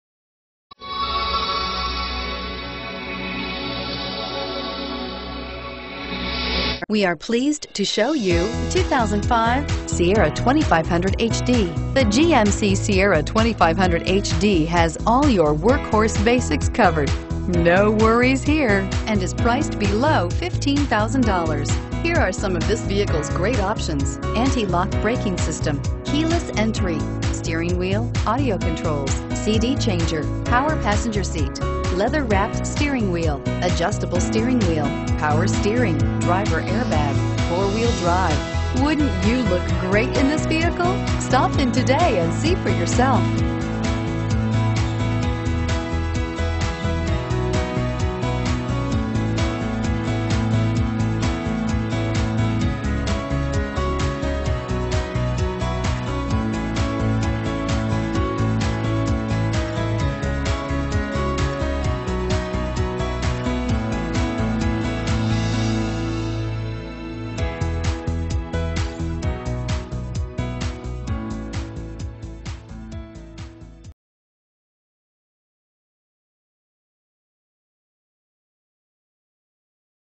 2005 GMC Sierra 2500HD Austin 349063A
2005 GMC Sierra 2500HD SLT

Looking for the right crew cab pickup? Today could be your lucky day. this 2005 GMC Sierra 2500HD could be yours.